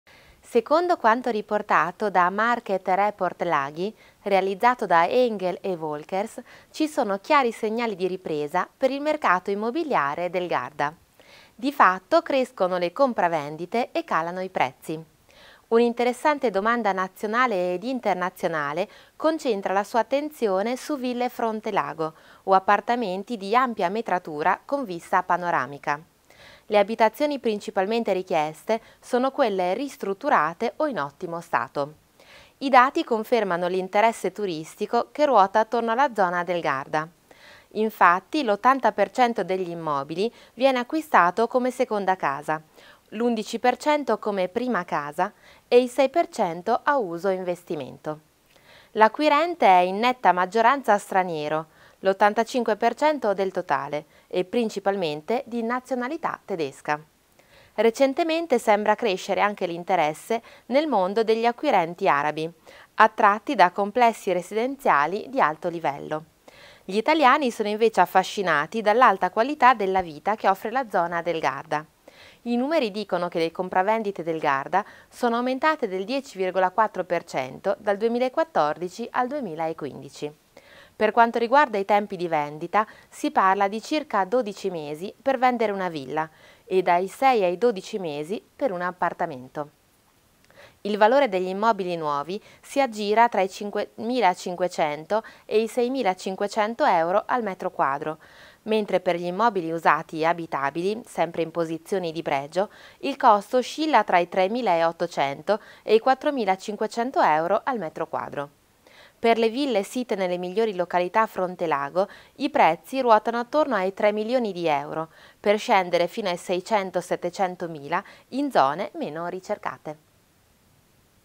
Segnali di ripresa per il mercato immobiliare del Garda
Servizio del telegiornale dell'11 febbraio 2017.

Il mercato immobiliare sul Garda nel 2016.